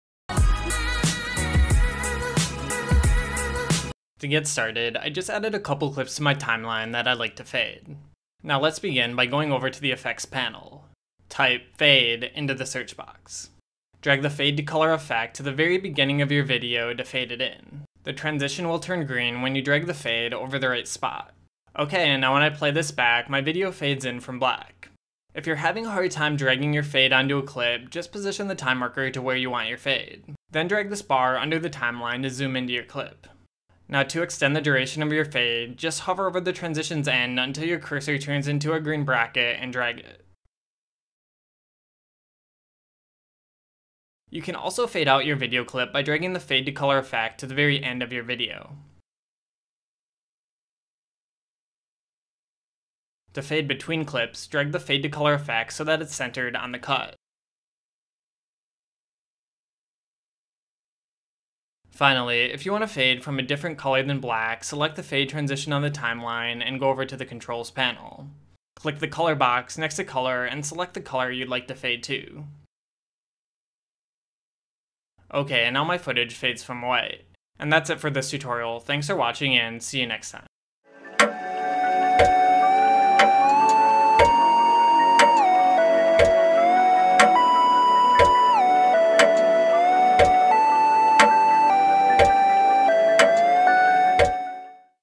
How to Fade in and Out Video in Hitfilm Express (Fast Tutorial)
A tutorial on how to fade in and fade out video in Hitfilm Express.